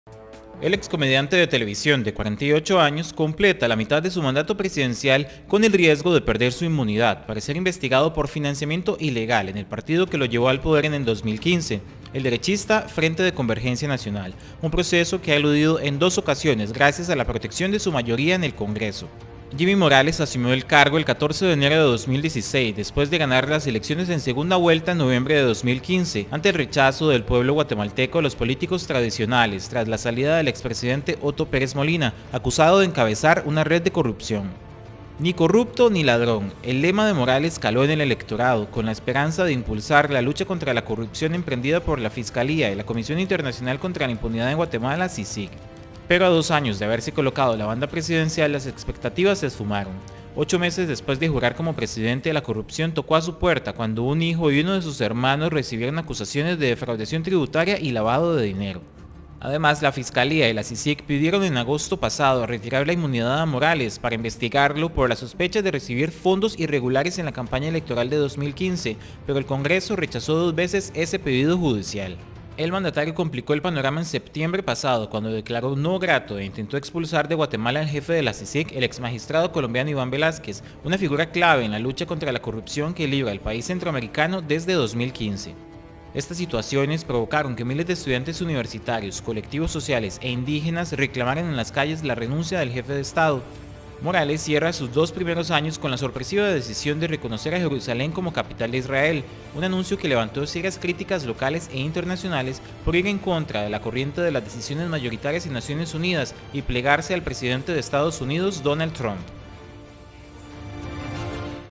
Presidente de Guatemala llega a la mitad de su mandato cuestionado por corrupción
El presidente de Guatemala, Jimmy Morales, completó el segundo de sus cuatro años de gobierno cuestionado por decisiones polémicas, como intentar expulsar al jefe de la CICIG, Iván Velásquez, que lo acusó de corrupción.

El excomediante de televisión de 48 años completa la mitad de su mandato presidencial con el riesgo de perder su inmunidad para ser investigado por financiamiento ilegal en el partido que lo llevó al poder en 2015, el derechista Frente de Convergencia Nacional, un proceso que ha eludido en dos ocasiones gracias a la protección de su mayoría en el Congreso.

Jimmy Morales asumió el cargo el 14 de enero de 2016 después de ganar las elecciones en segunda vuelta en noviembre de 2015, ante el rechazo del pueblo guatemalteco a los políticos tradicionales tras la salida del expresidente Otto Pérez, acusado de encabezar una red de corrupción.

"Ni corrupto ni ladrón", el lema de Morales caló en el electorado, con la esperanza de impulsar la lucha contra la corrupción emprendida por la fiscalía y la Comisión Internacional Contra la Impunidad en Guatemala (CICIG).

Ocho meses después de jurar como presidente, la corrupción tocó su puerta cuando un hijo y uno de sus hermanos recibieron acusaciones de defraudación tributaria y lavado de dinero.

Además, la Fiscalía y la CICIG pidieron en agosto pasado retirar la inmunidad a Morales para investigarlo por las sospechas de recibir fondos irregulares en la campaña electoral de 2015, pero el Congreso rechazó dos veces ese pedido judicial.

El mandatario complicó el panorama en setiembre pasado cuando declaró no grato e intentó expulsar de Guatemala al jefe de la CICIG, el exmagistrado colombiano Iván Velásquez, una figura clave en la lucha contra la corrupción que libra el país desde 2015.

Estas situaciones provocaron que miles de estudiantes universitarios, colectivos sociales e indígenas reclamaran en las calles la renuncia del jefe de Estado.

Morales también cierra sus primeros dos años con la sorpresiva decisión de reconocer a Jerusalén como capital de Israel, un anuncio que levantó serias críticas locales e internacionales por ir contra la corriente de las decisiones mayoritarias en Naciones Unidas y plegarse al presidente de Estados Unidos, Donald Trump.

Redacción / CB24